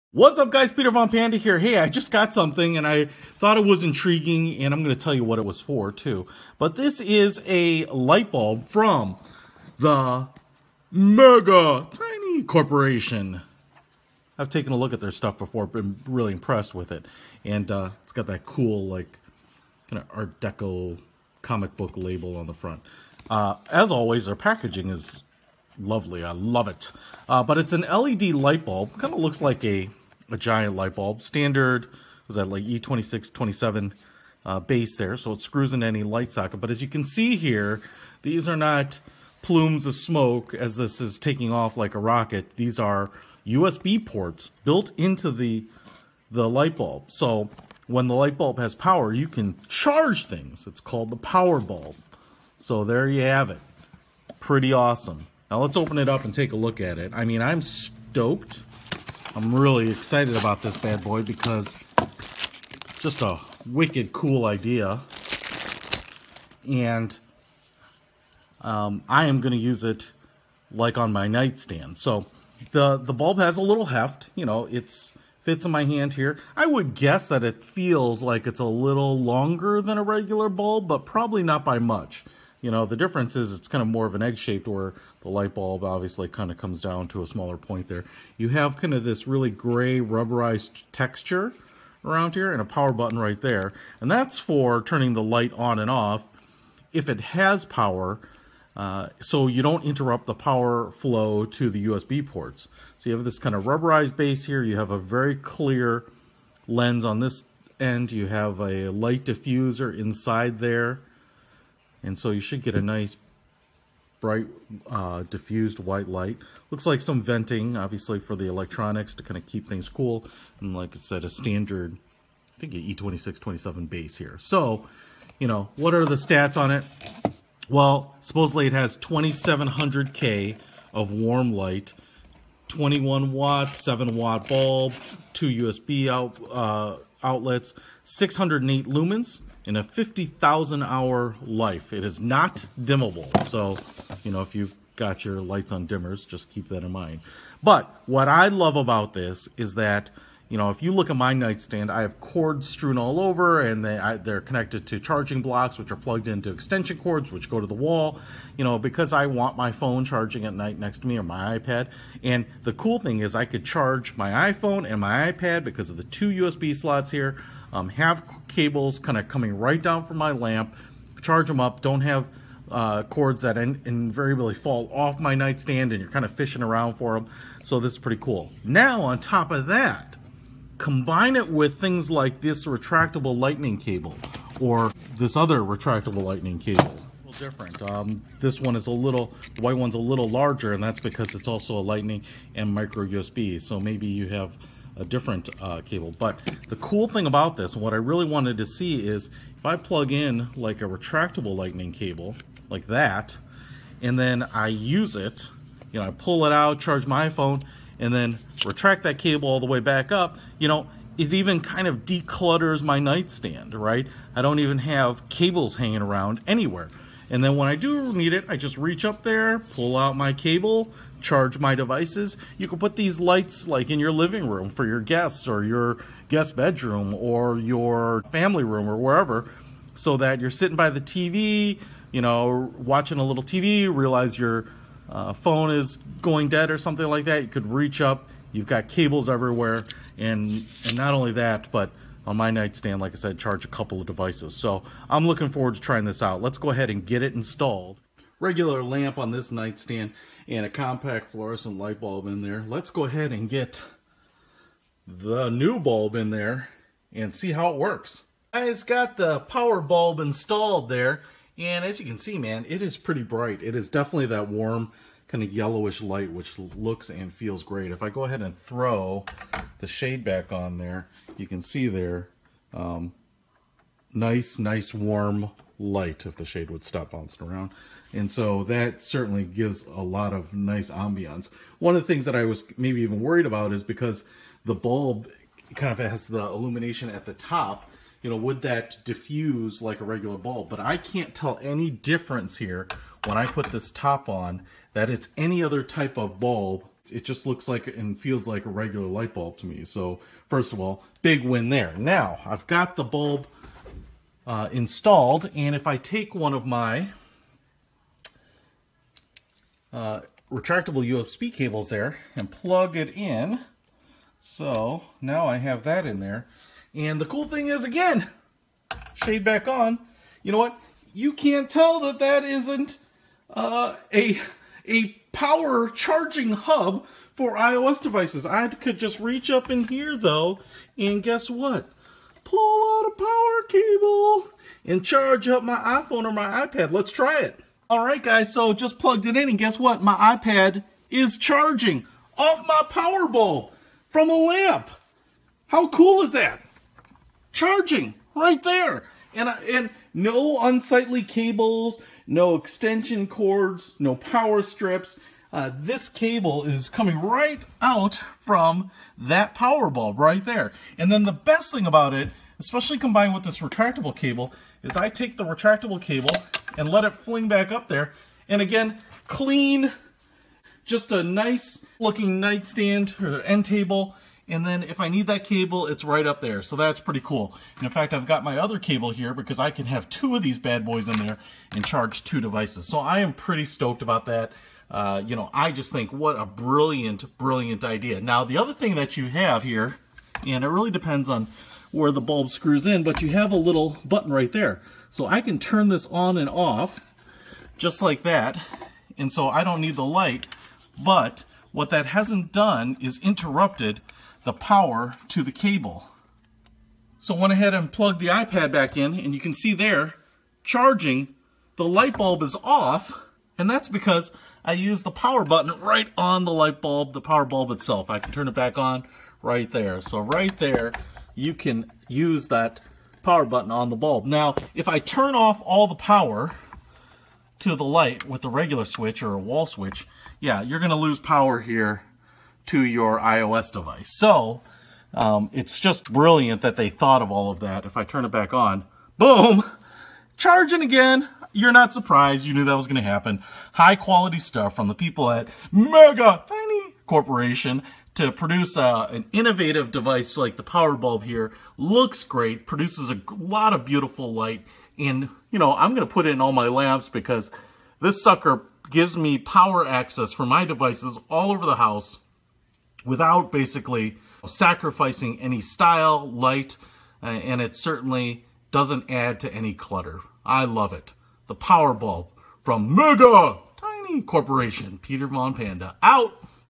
Charge a USB Device from a light bulb with the PowerBulb from Mega Tiny Corp
LIGHT MEETS POWER™
PowerBulb is the world's first LED light bulb with dual USB ports that turns any lamp into a charging station.

Charge Any USB Device From A Light Bulb

PowerBulb is the world's first LED light bulb with dual USB ports that turns any lamp into a charging station. We created the PowerBulb as a simple, elegant solution to the problems we face when looking for convenient ways to charge our devices.

With MegaSmart™ Charging, PowerBulb automatically senses and sends more power to the device with the lowest charge. And the dedicated on/off button on the bulb allows you to turn the light off while continuing to charge your devices.

No more extension cords under the couch or under the bed, no more losing charging adapters, no more power strips everywhere. The PowerBulb will charge any 2 devices simultaneously including iPads, tables, smartphones, watches, Kindles, or any accessory that can be charged via USB charging cables.

Warm Soft White LED Bulb
608 Lumens
2700 degrees Kelvin
Weight: 0.6lbs
Height: 5.9in
Width 2.8in
Works with All E26 & E27 Sockets
Polycarbonite Lens
LED light with soft light diffuser
Cast Aluminum Heat Sink
Soft Touch Power Button
2 USB Ports
50,000 Hour Bulb Life Expedition Money LLC
A Financial Wellness Program That Works